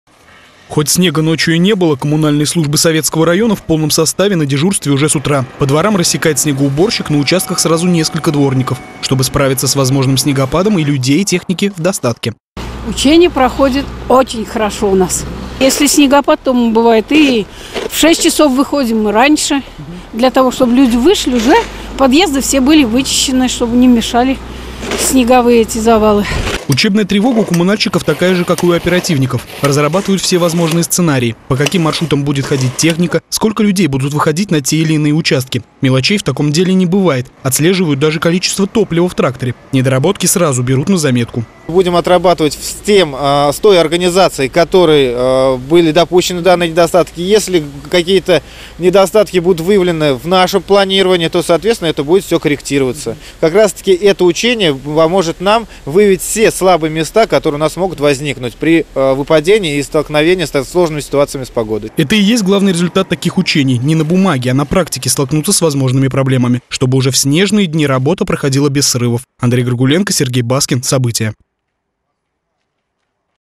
Коммунальная учебная тревога. Самарские работники ЖКХ отработали действия при сильном снегопаде
На небе ни облачка, но в Самаре объявили снегопад 4-ой категории. Это учебная тревога не для горожан, а для коммунальных служб. В городе проверяют, насколько управляющие компании  готовы к капризам природы. Какую оценку заслужили бойцы лопаты и метлы.